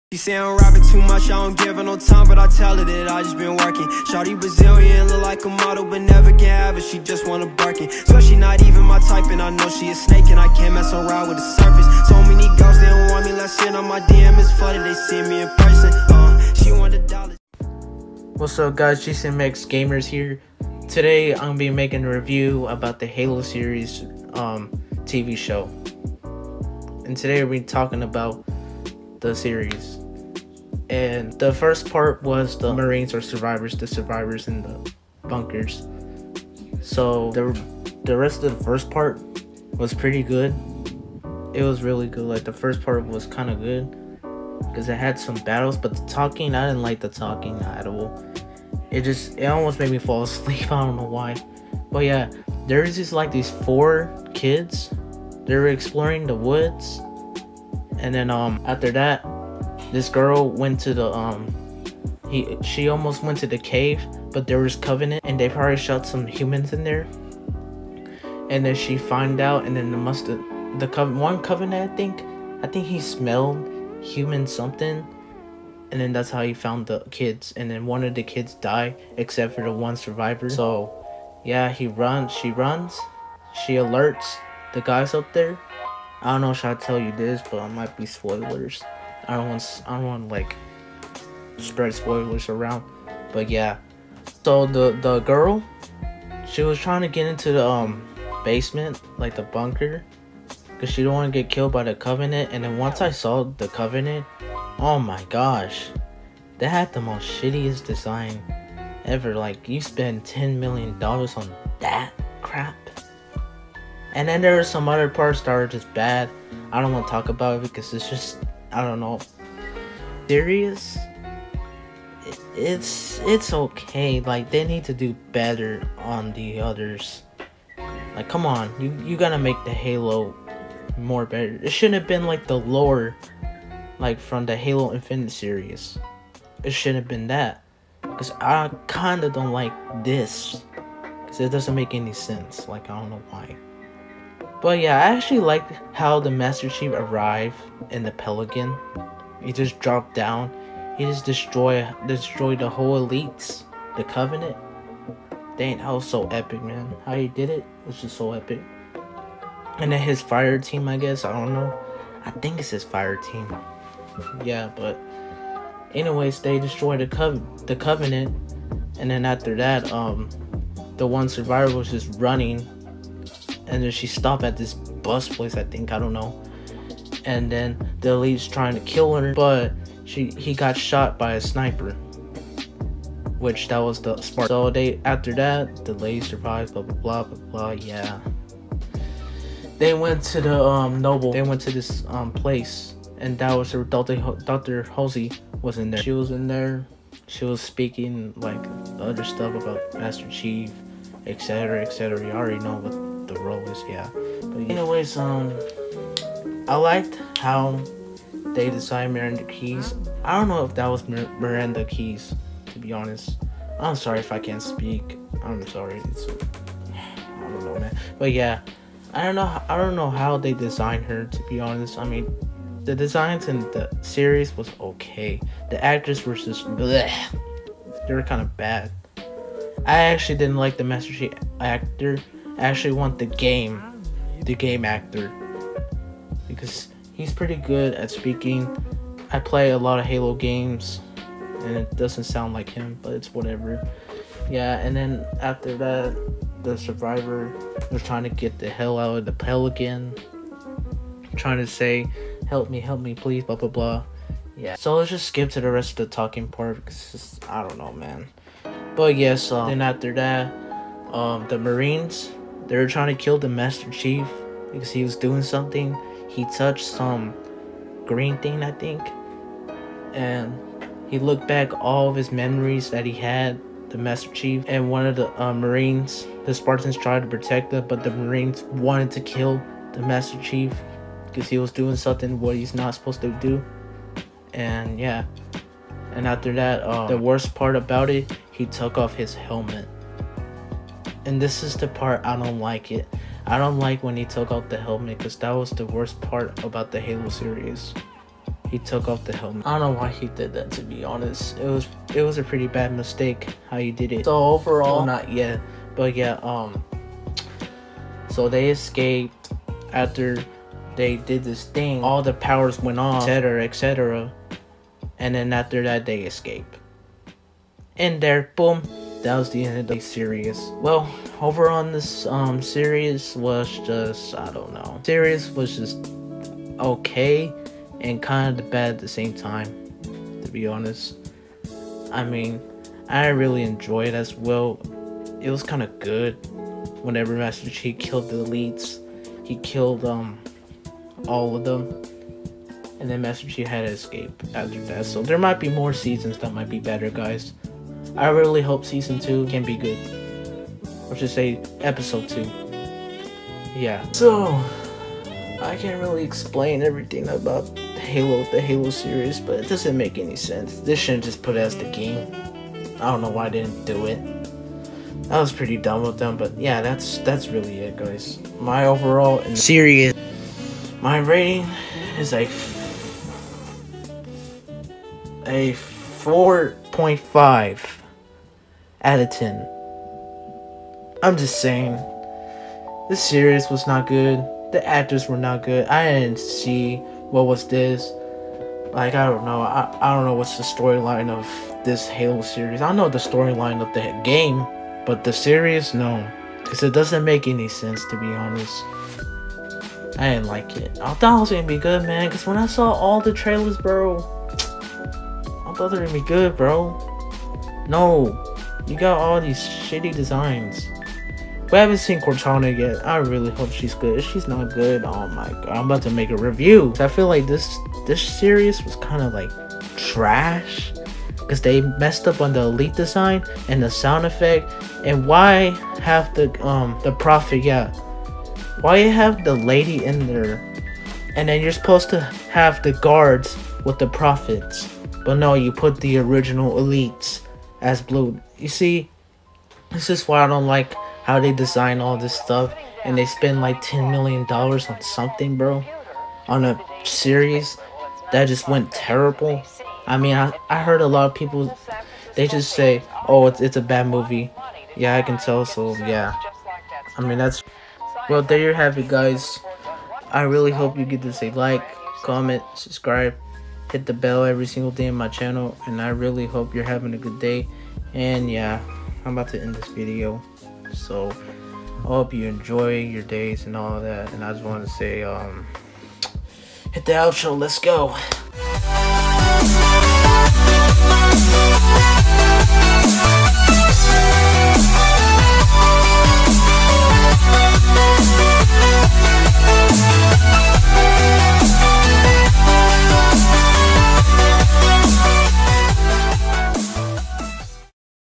HALO TV SERIES REVIEW BAD FOR MY HONEST THOUGHTS
COMMENT & BELL
PLEASE🙏🏽
So I have made a halo Tv series review I just got done watching it yesterday it was pretty bad I didn’t like how the elite design it was so bad and yeah I am going to explain in this video and I hope you enjoy this so see ya in the next one also please go these in the description

Go do those right now and remember stay tune for some more guys